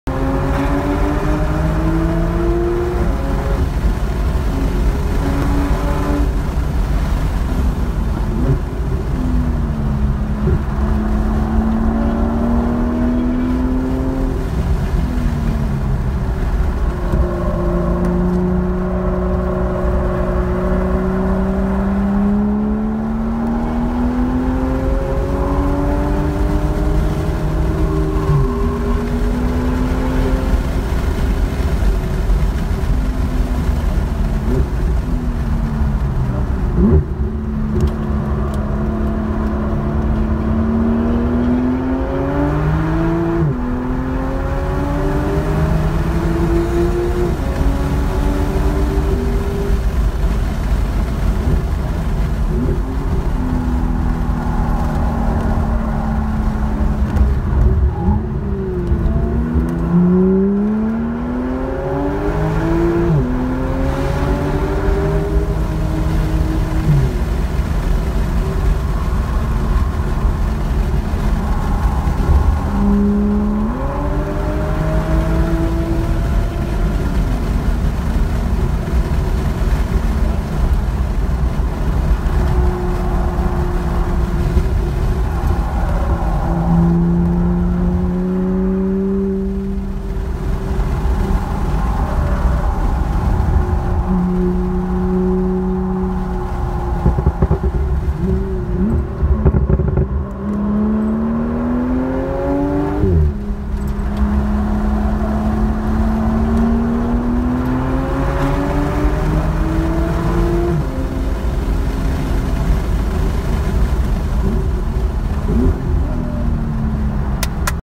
Just Track It Barber Motorsports Park Fastest Lap 20210822
This clip was from my 8th session in the Novice (Green) Solo. First time using Harry's Lap Timer. Chasing a GT3. Not sure what the segments mean or the countdown. Also, I paired it with my obd II sensor but I think it's off. Regardless, this was my fastest lap at Barber Motorsports Park.